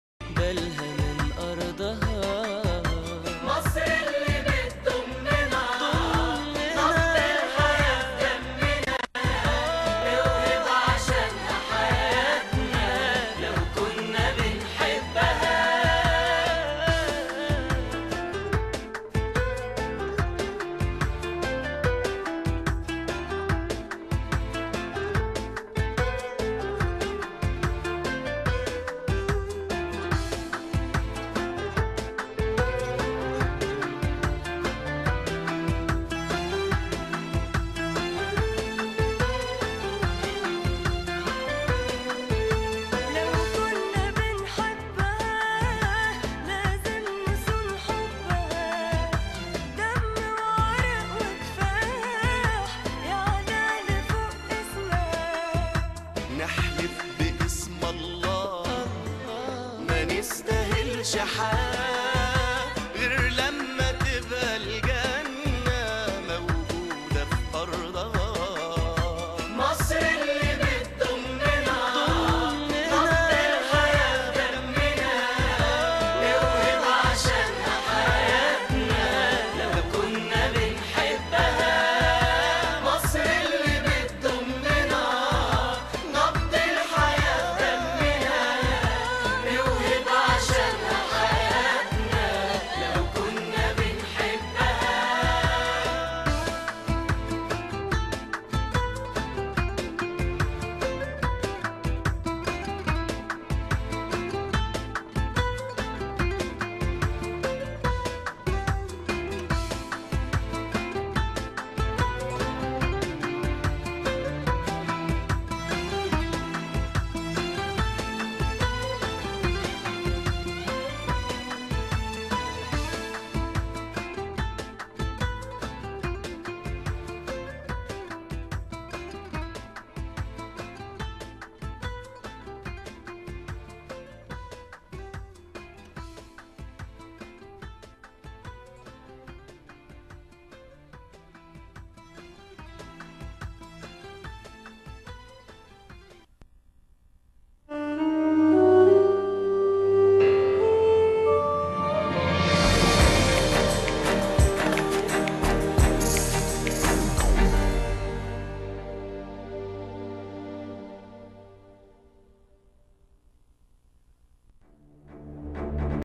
Patriotic song on state run Nile TV
program from Egypt during the riots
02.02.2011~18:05